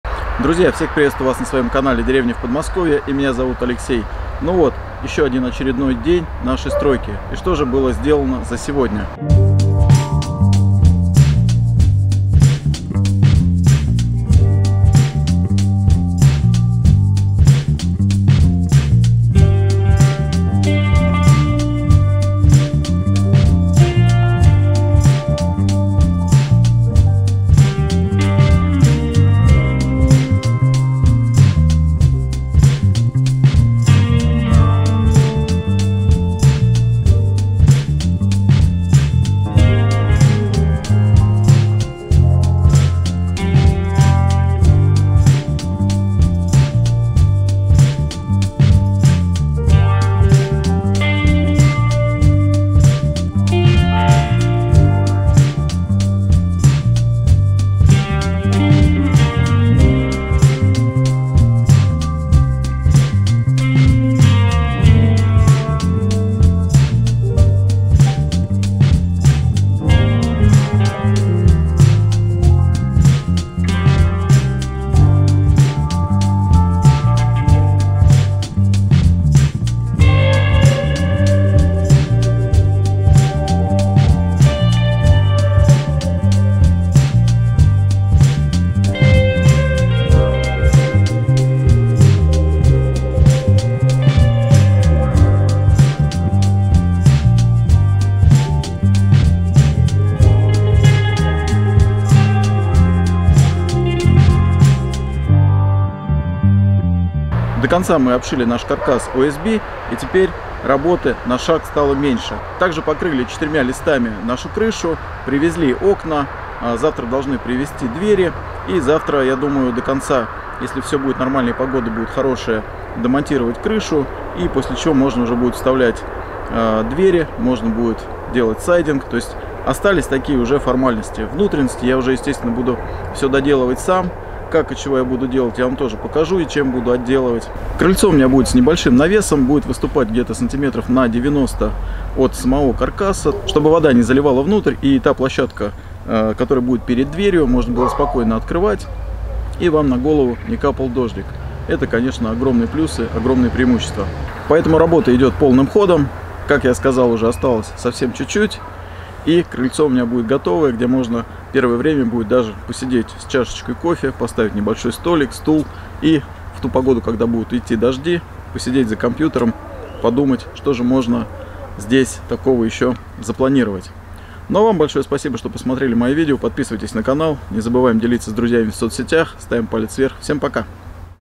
Деревянное крыльцо для дома в деревне, зашил OSB, крою крышу
Деревянное крыльцо к моему дому в деревне все больше становится похожим на крыльцо. Основная работа сделана, остаются нюансы. Закрываю OSB, крою крышу.

🏡 ОСТАЛИСЬ ВОПРОСЫ? – Пиши комментарий.